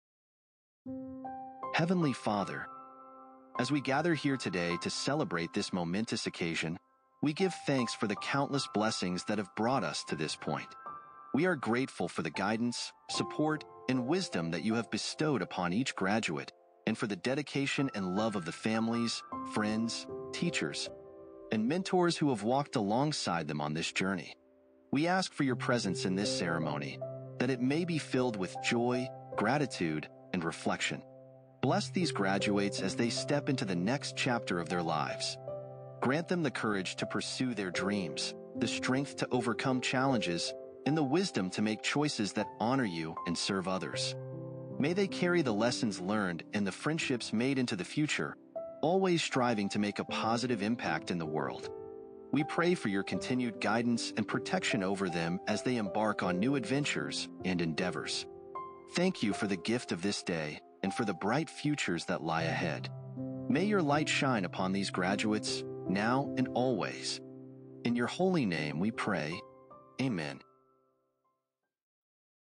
Opening Prayer for Graduation Day High School and College
🎓✨ Today, we celebrate a milestone! To all the graduates, may your journey ahead be filled with courage, wisdom, and endless opportunities. Your hard work and dedication have brought you to this moment, and we couldn't be prouder. Here's to new beginnings and the bright futures that await! 🌟🙏